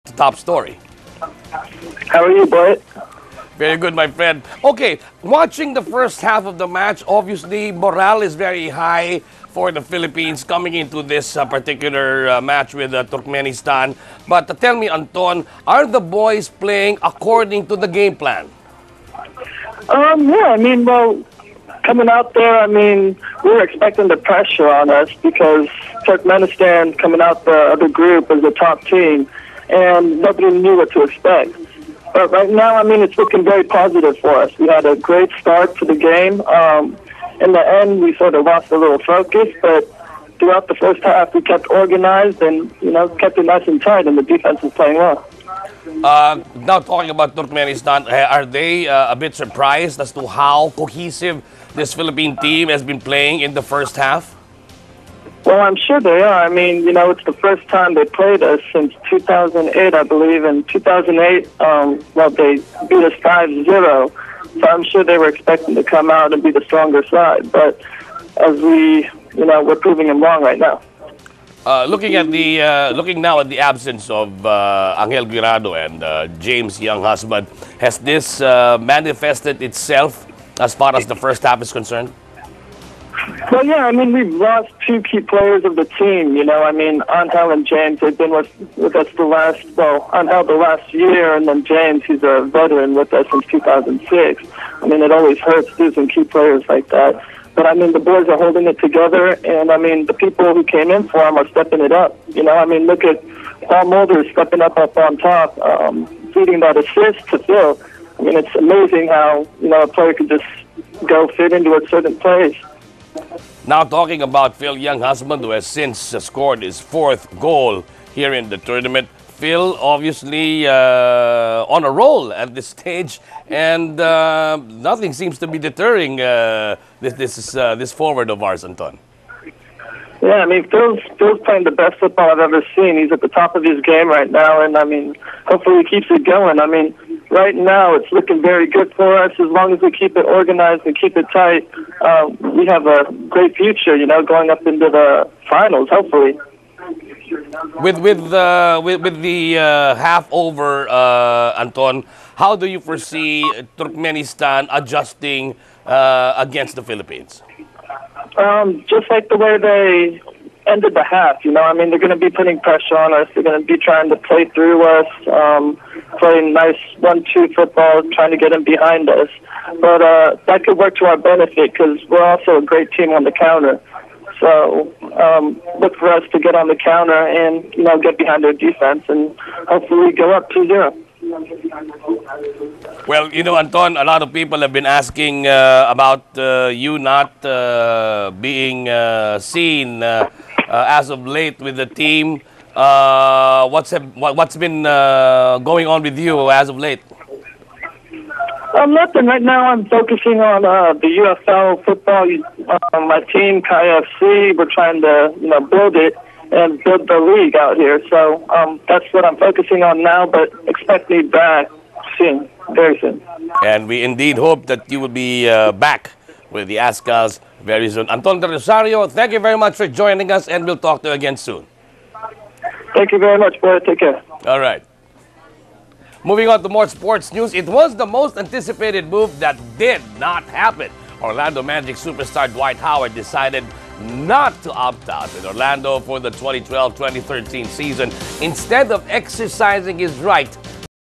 Highlights of the first half of PH vs TKM game at AFC Challenge Cup
Azkals member Anton del Rosario talks to ANC, helps us break down first half of AFC Challenge Cup semis"